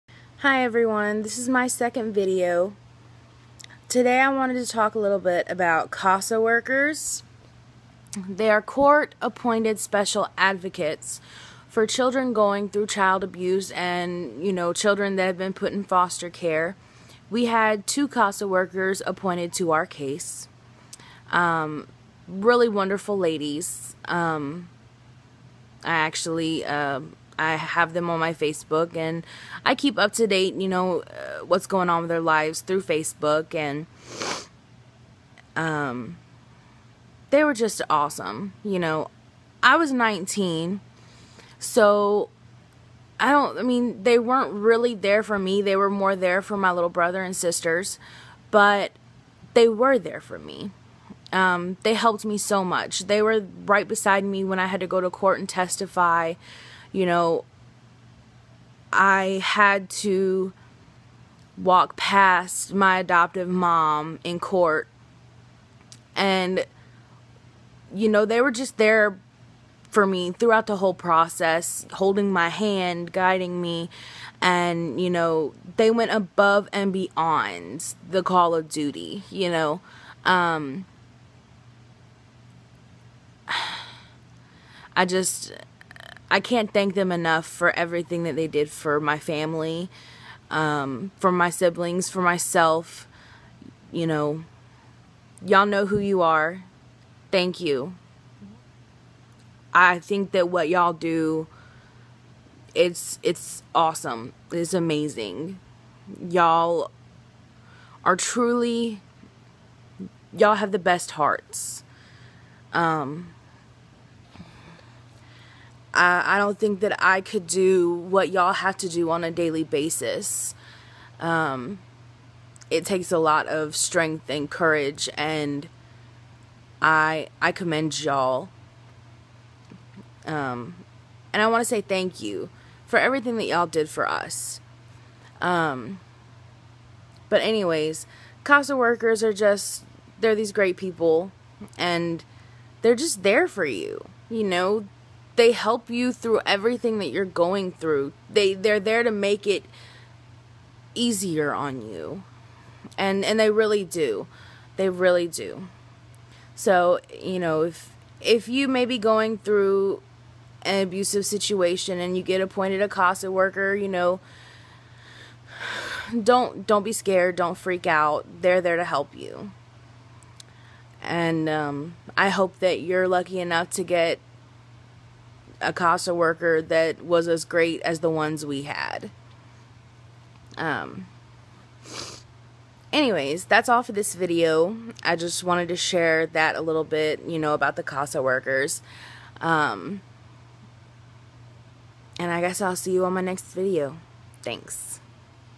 CASA Workers!
CASA means Court Appointed Special Advocate! They help children who have been placed in foster care!